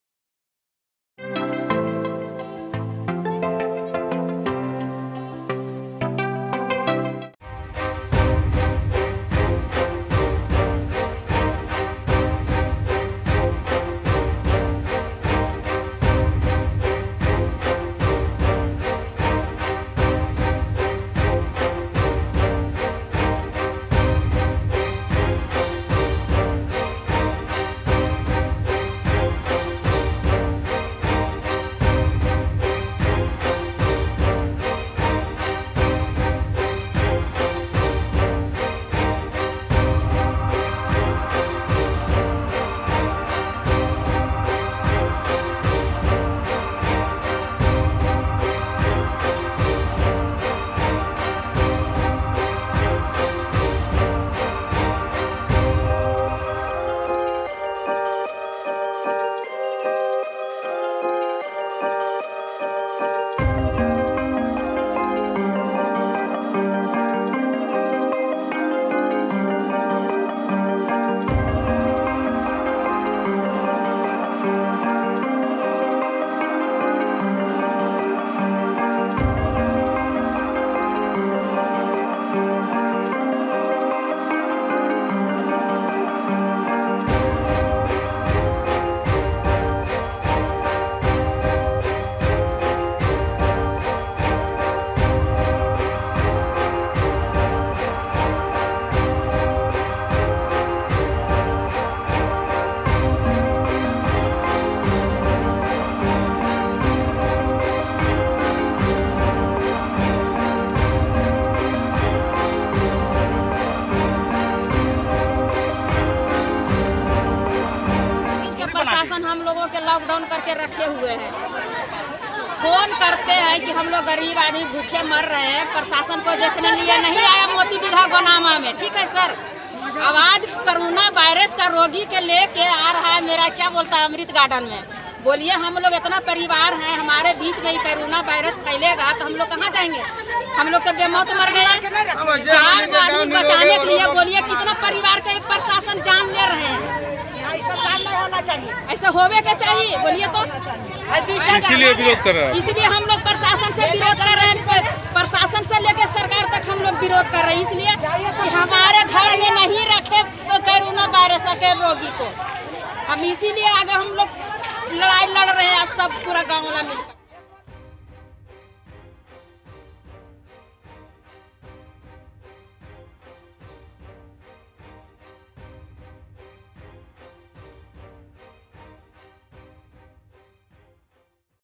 Nawada: होटल को क्वारंटाइन सेंटर बनाने पर ग्रामीणों ने किया जमकर विरोध
नवादा के होटल को क्वारंटाइन सेंटर बनाने का ग्रामीणों ने किया विरोध. ग्रामीणों बोले - इस होटल में नहीं रुकने देंगे कोरोना पॉजिटिव मरीजों को ग्रामीणों ने नहीं सुनी एसडीओ, डीएसपी,एएसपी की बात नवादा के अमृत गार्डन होटल को बनाया जा रहा था क्वारंटाइन सेंटर.